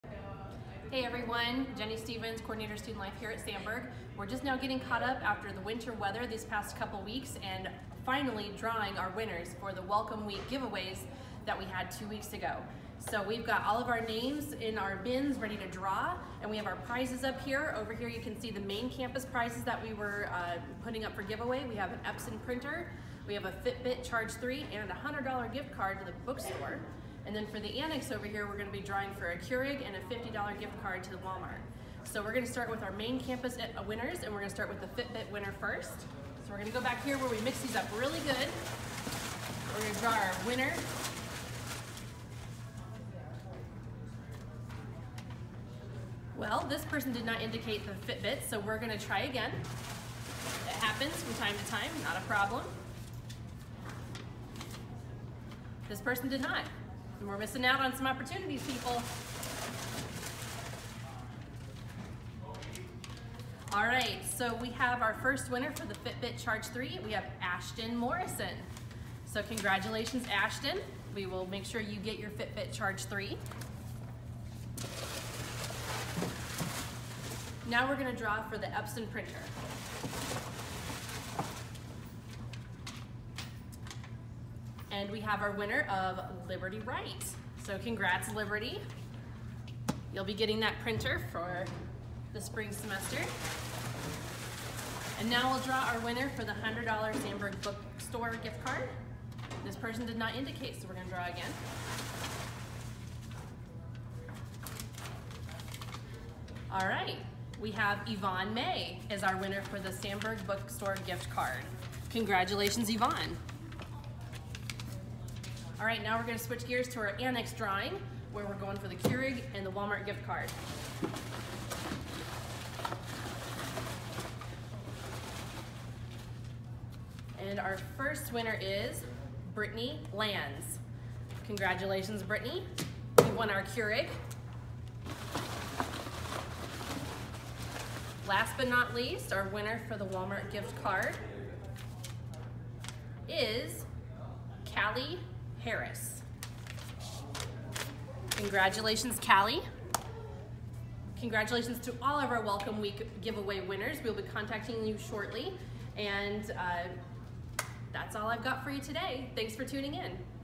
Welcome Week Raffle Winners Spring 2019
Congratulations to our Welcome Week raffle winners!

Main Campus Drawings:
• Yovnne May - $100 Sandburg Bookstore Gift Card 💳
• Ashtyn Morrison - Fitbit Charge 3 ⌚️
• Liberty Wright - Epson XP-466 Printer 🖨

Annex Drawings:
• Brittany Lanz - Keurig ☕️
• Kalli Haris - Walmart Gift Card 💳